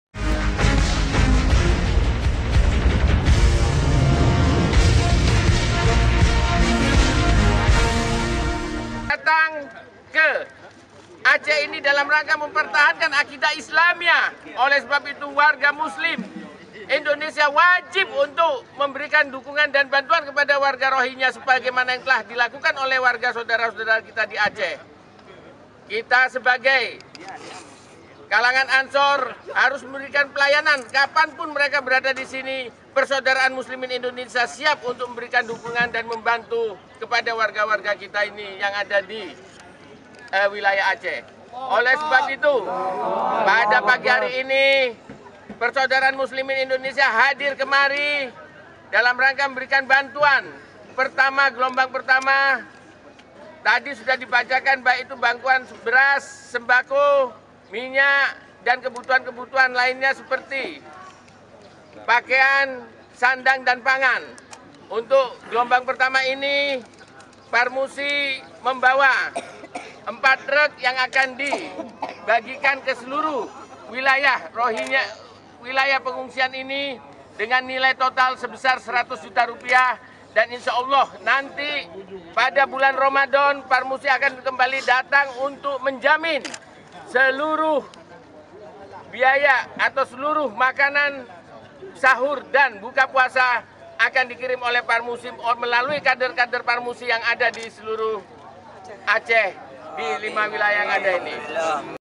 Parmusi Bantu Pengungsi Rohingya
acehbaru.TV-acehbaru.TV- Dewan  Pengurus Pusat (DPP)  Persaudaraan Muslimin Indonesia (Parmusi) mengantar bantuan sandang dan pangan untuk 332 pengungsi Rohingya, yang ditampung di Komplek Tempat Pendaratan Ikan (TPI) Kuala Cangkoi, Kec. Lapang, Aceh Utara, Jumat 29 Mei 2015. parmusi juga mengantar bantuan logistik ke Kamp pengungsi Eks Imigrasi Punteut, Pengungsi Bayeun, Langsa dan Aceh Tamiang